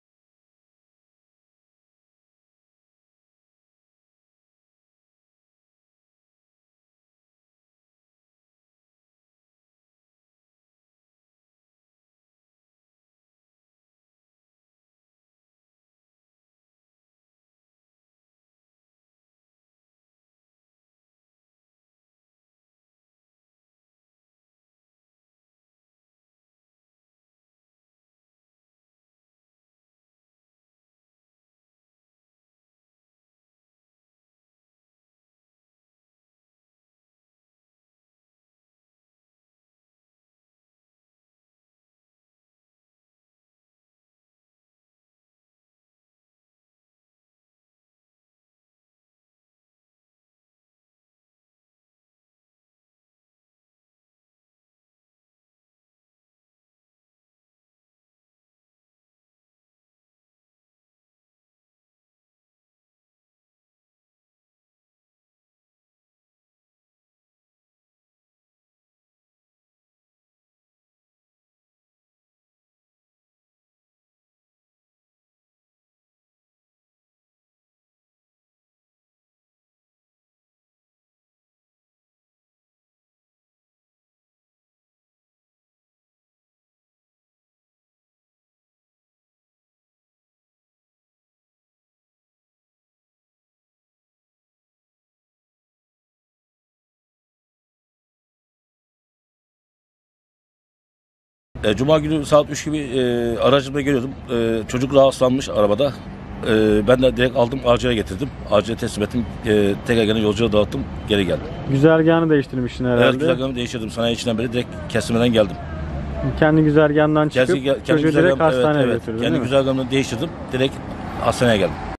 Fenalaşan genci minibüs şoförü hastaneye yetiştirdi
DÜZCE (İHA) – Düzce’de minibüse binen yolculardan birisi rahatsızlanınca, minibüs sürücüsü hattından çıkarak ambulans gibi kullandığı minibüs ile yolcuyu hastaneye yetiştirdi.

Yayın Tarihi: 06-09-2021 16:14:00